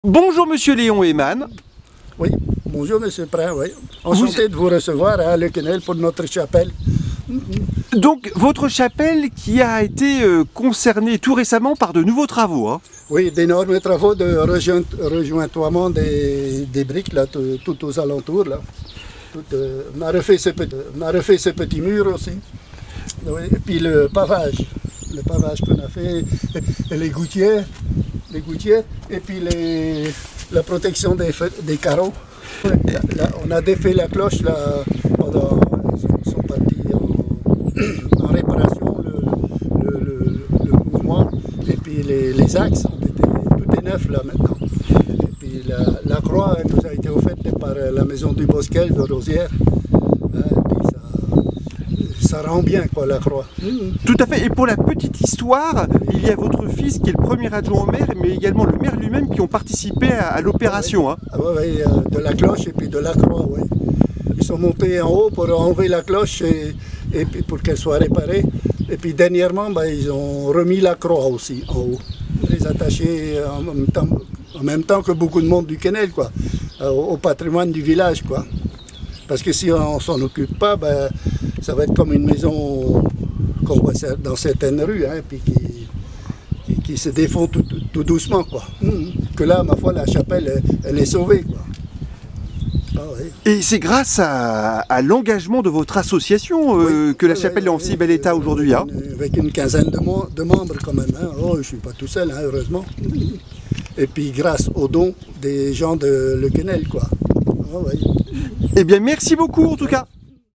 Somme : la chapelle du Quesnel retrouve sa cloche et une croix
Grâce à l'engagement de l'association Le Quesnel Patrimoine et Traditions, de la mairie, et à une subvention du Département de la Somme et à un partenariat avec la Fondation du patrimoine, la chapelle Notre-Dame-Auxiliatrice a bénéficié de nombreux soins ces dernières années : murs rejointoyés, vitraux protégés, parvis refait avec de nouvelles dalles, gouttières changées. Et, plus récemment, en février dernier, l'édifice a retrouvé sa cloche et une croix la surplombe désormais, offerte par l'établissement de pompes funèbres Dubosqueille de Rosières-en-Santerre.